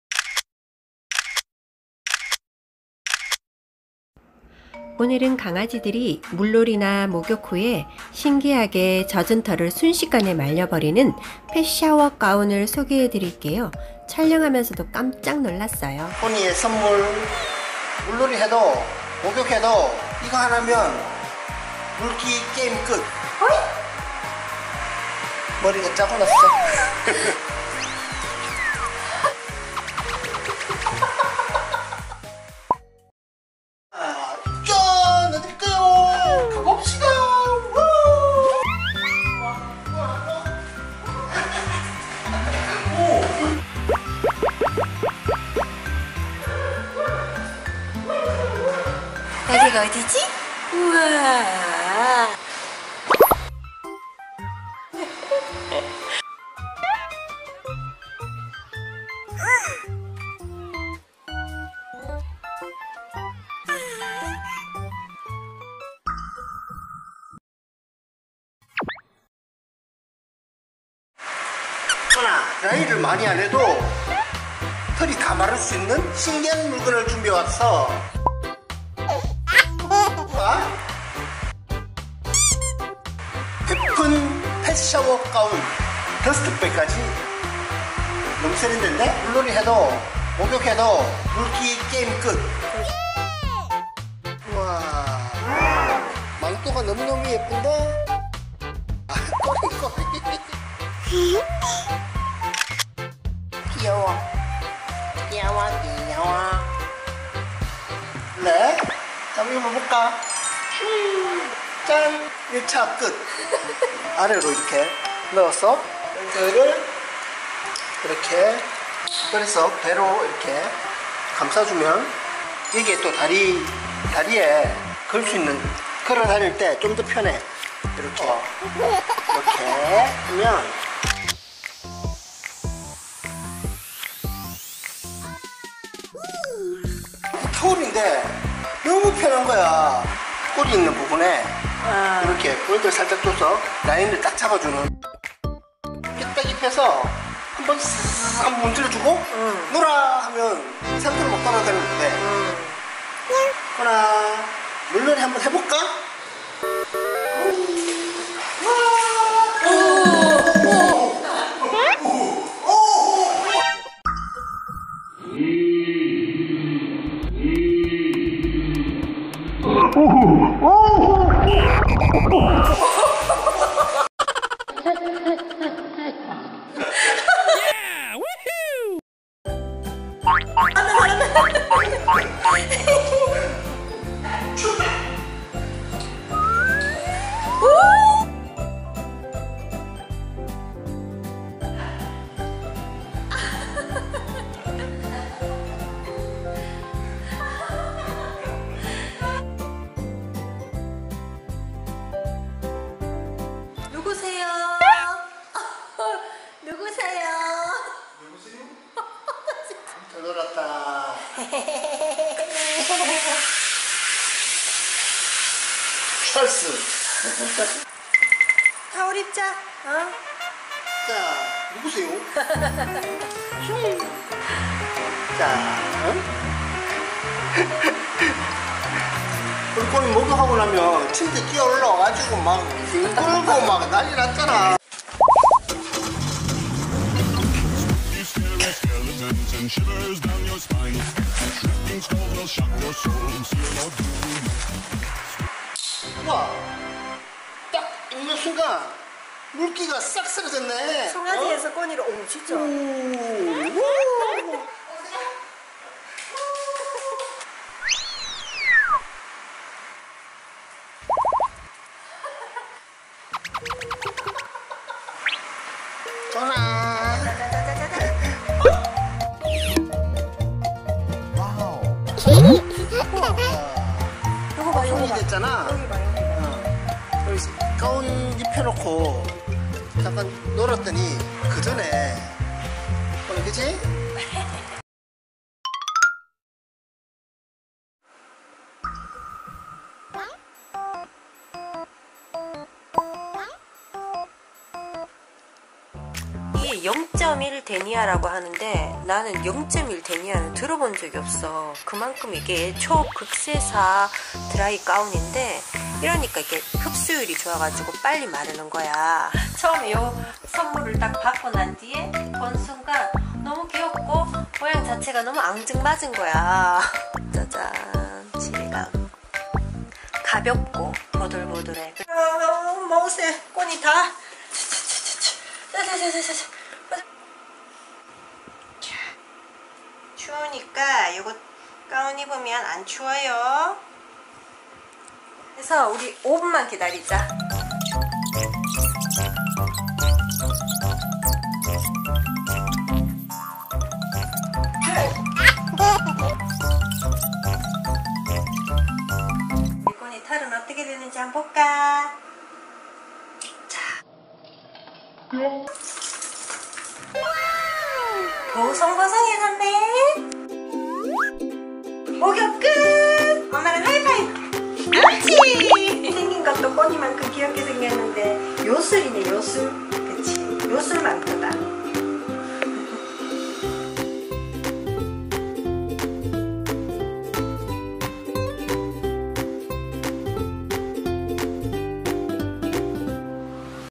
까불다 수영장에 빠져버린 강아지에게 가운을 입혀줬더니 3분만에 털이 다 말라버렸어요! (feat. 깜찍한 베픈 펫샤워가운.. 넘 귀엽잖아!ㅎㅎㅎ)
강아지와 함께 살고있는 가정이라면 물놀이, 목욕후 3분이면 물기가 70%이상 말라버려서 강아지가 스트레스 받지않고 털을 말릴수 있는 베픈 펫샤워가운 하나 꼭 준비하세요!
입어보니 꼬니가 훨씬 더 귀여워졌어요. 넘넘 귀여워요!ㅎㅎㅎㅎ
수영장 갈때! 물놀이 갈때! 집에서 목욕할때! 정말 강추드려요!^-^

본 영상의 "베픈 펫샤워가운"의 구매링크입니다.

Subtitles are being serviced. Please turn on the subtitles.^^